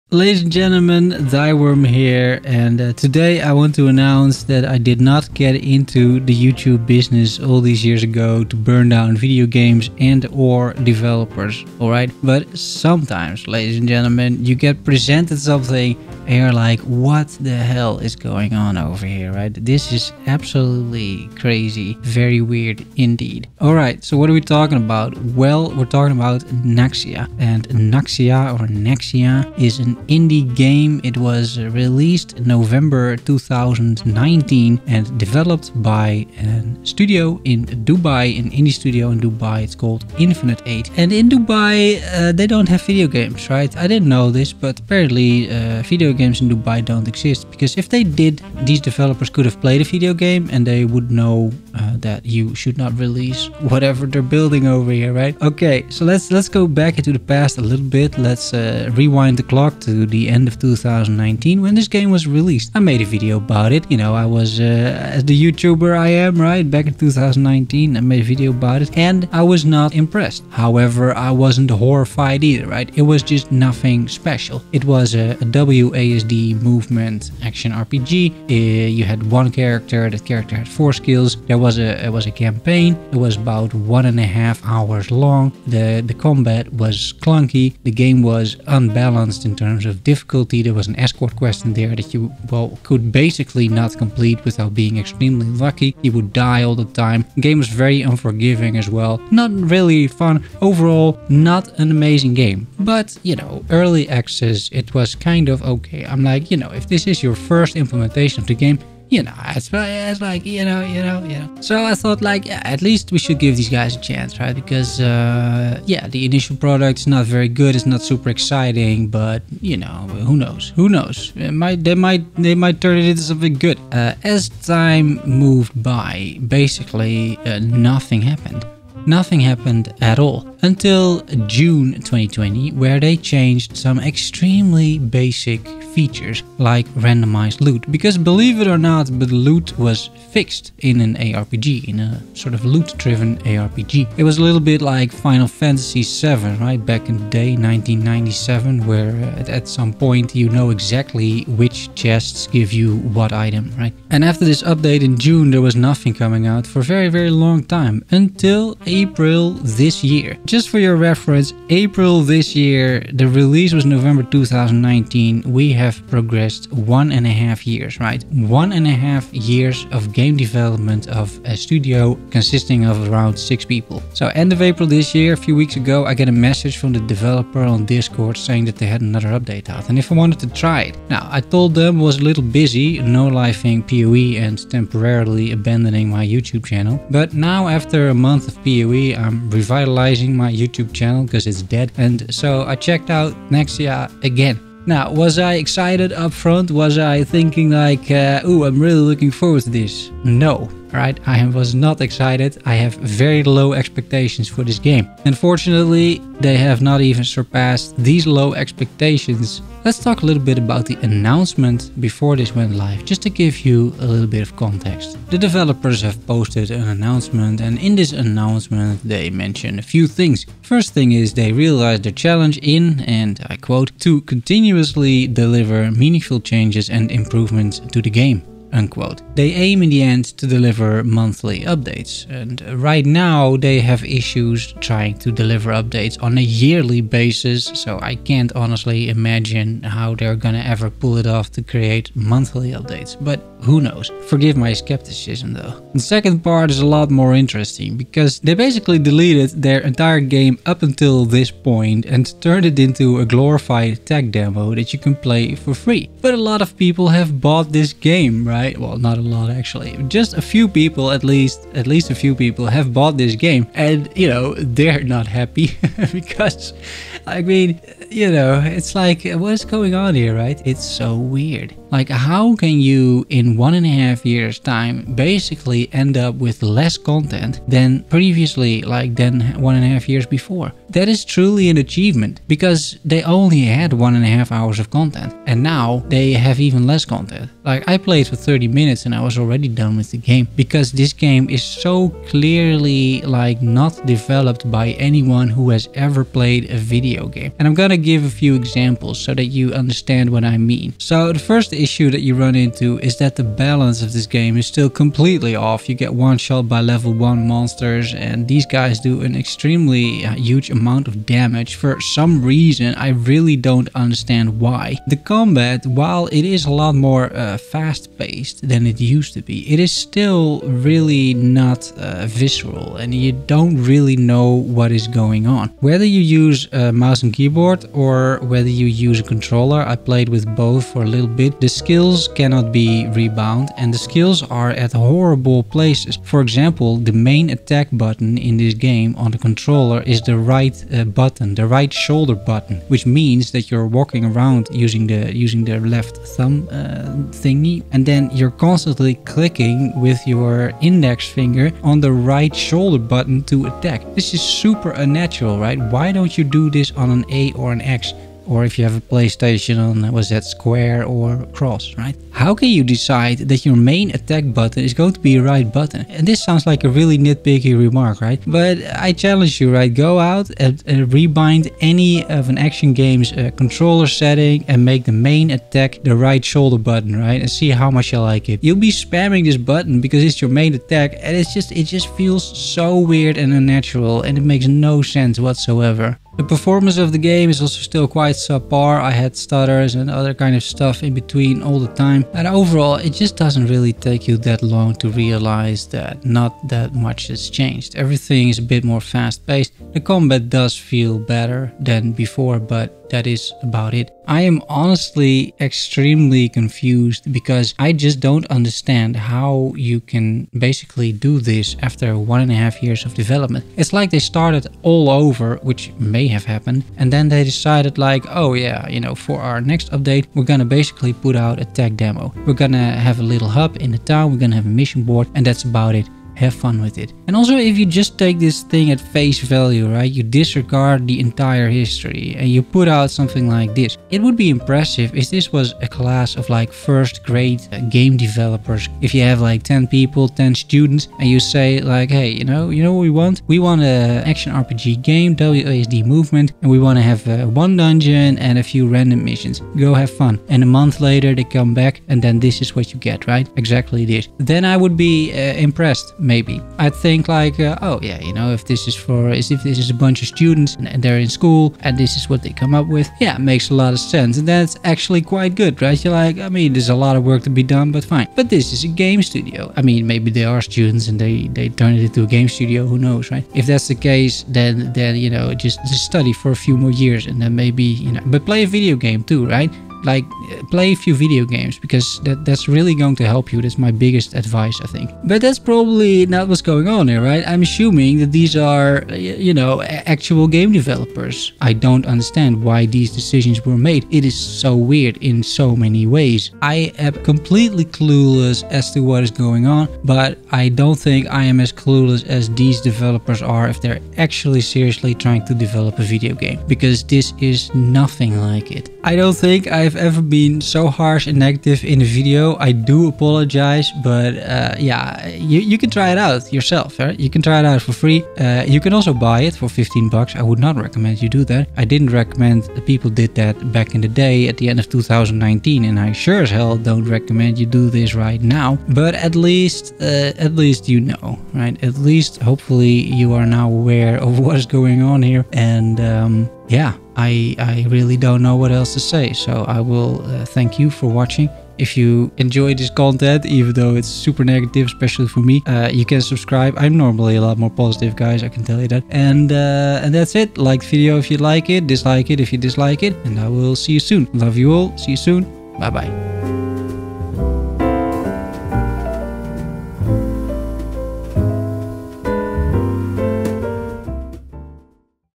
Naxia has less content than 1,5 years ago
I'm not sure what's going on with this game, but Naxia recently overhauled their game and suffice to say, there weren't many improvements. It has less content now than 1,5 years ago. I'm clueless. Curious about your thoughts.

*** Follow the live streams ***
Right here on YouTube, check out the planned events!